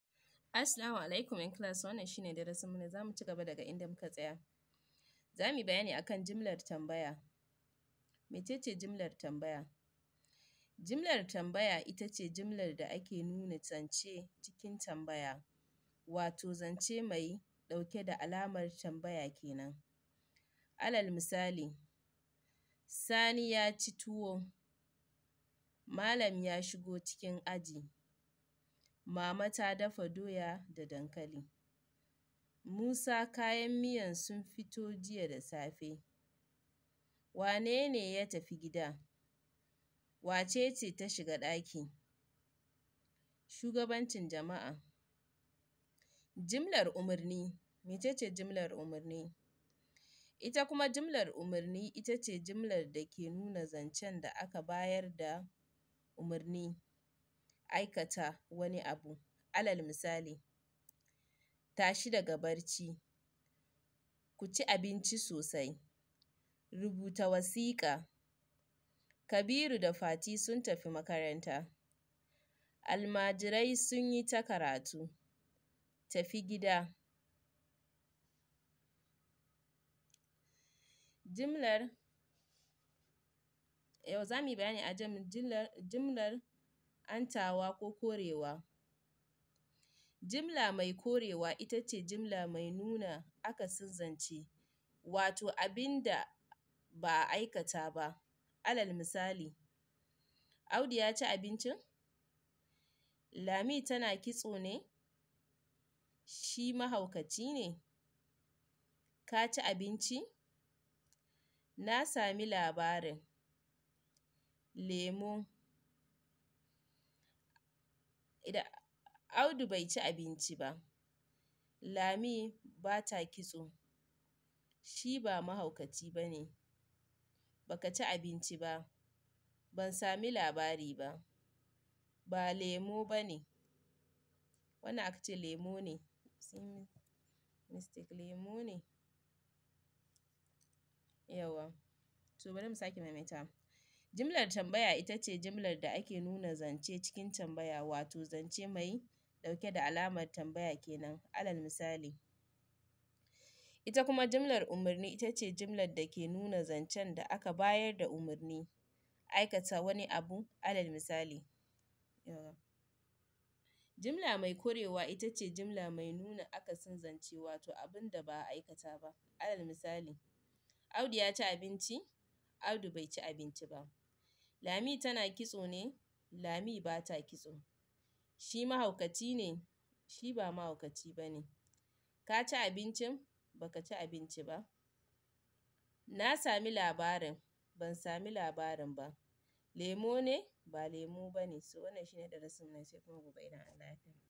Darussan hausa a kammale karantawa zeebappahtv in 2023
published on 13/3/2023 wanna video Yana maganane akan jimla tanbaya da misallansu